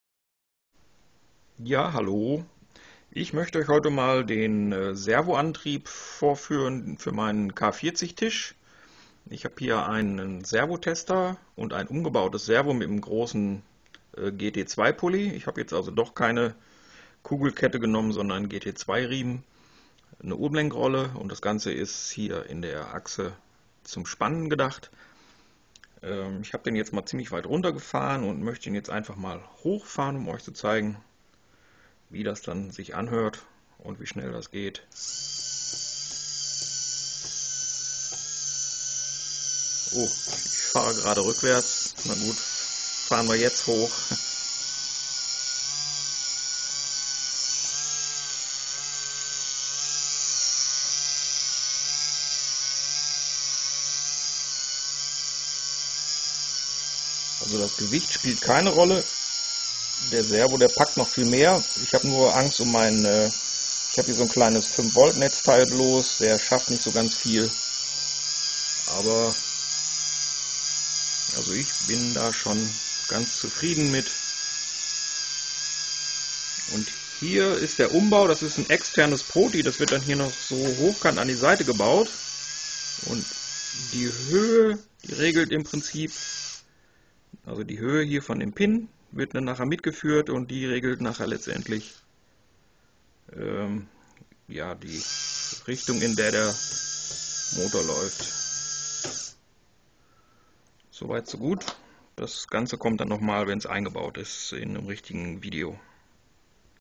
servodriven table for K40 laser
Instead of a heavy and bulky NEMA17, I decided to use a hobby servo to drive the table. The servo is modified with an external linear potmeter to get closed loop control.
Later, the servo signal will come from a Raspberry Pi.